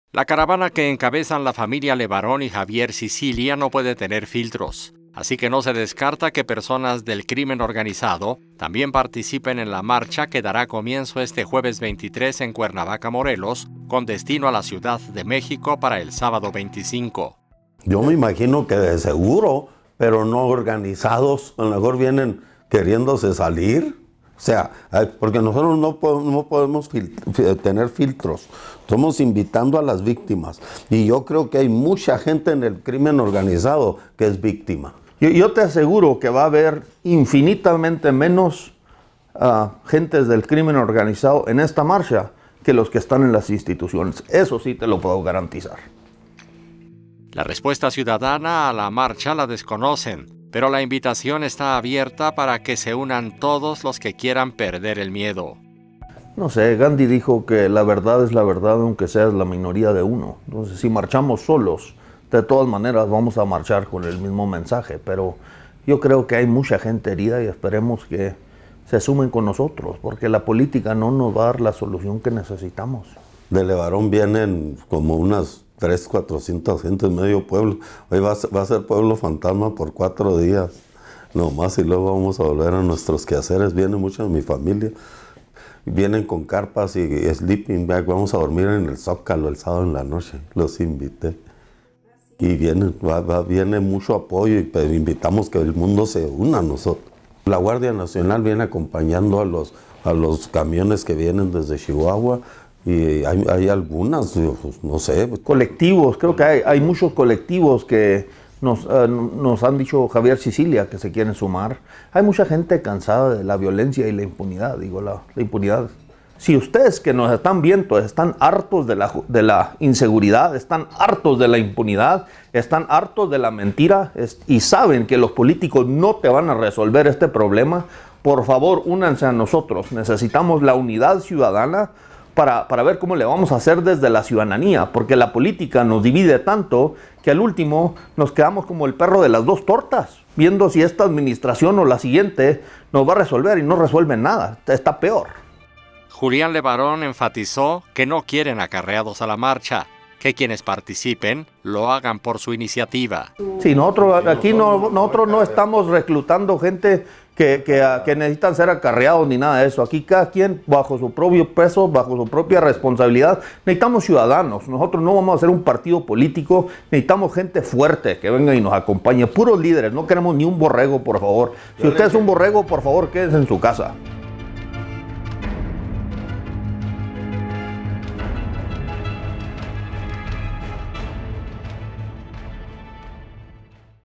Dormiremos en Zócalo de la CDMX: LeBarón
La caravana que encabezan la familia LeBarón y Javier Sicilia no puede tener filtros, así que no se descarta que personas del crimen organizado también participen en la marcha que dará comienzo este jueves 23 en Cuernavaca, Morelos con destino a la Ciudad de México para el sábado 25.